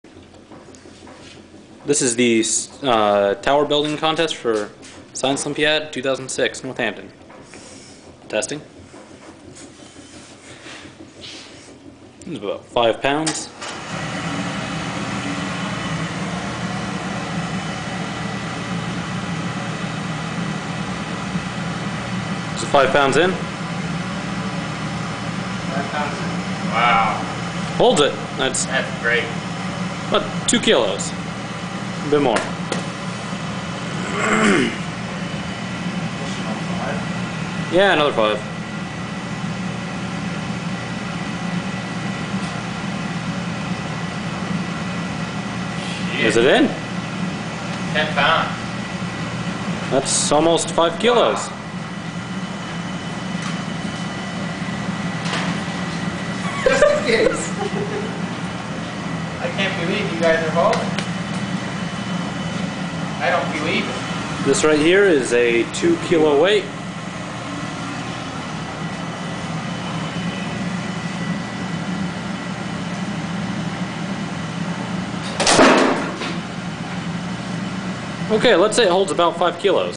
Science Olympiad Tower Test 2
Dan testing preliminary balsa tower for Science Olympiad Competition